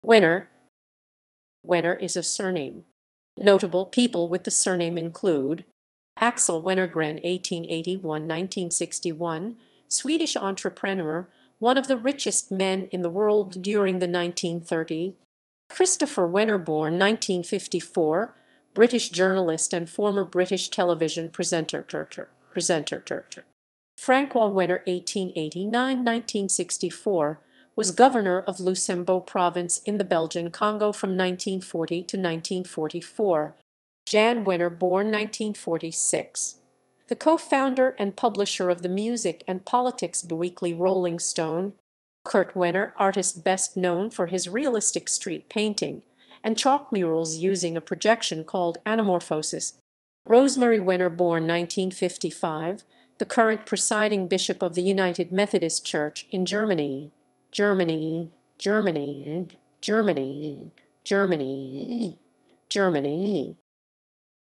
Wenner
Wenner is a surname. Notable people with the surname include:

Axel Wenner-Gren (1881–1961), Swedish entrepreneur, one of the richest men in the world during the 1930s
Christopher Wenner (born 1954), British journalist and former British television presenter
François Wenner (1889–1964), was governor of Lusambo Province in the Belgian Congo from 1940 to 1944
Jann Wenner (born 1946), the co-founder and publisher of the music and politics biweekly Rolling Stone
Kurt Wenner, artist best known for his realistic street painting and chalk murals using a projection called anamorphosis
Rosemarie Wenner (born 1955), the current Presiding Bishop of the United Methodist Church in Germany